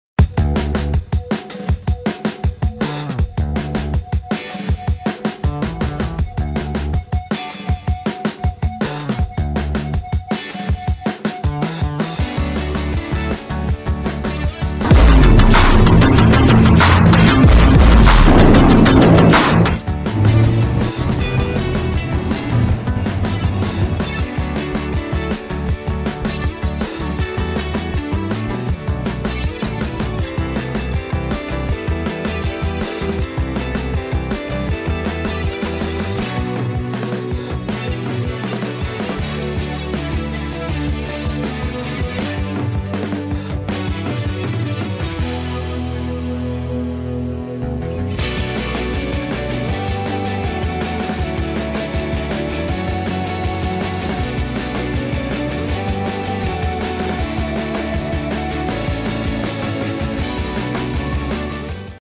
Cet africain défie Jason Statham ! 💪🏾
Le comédien nigérian BasketMouth est le meilleur au BottleCapChallenge ! 👑
★ Suivez Wada7Afrique sur :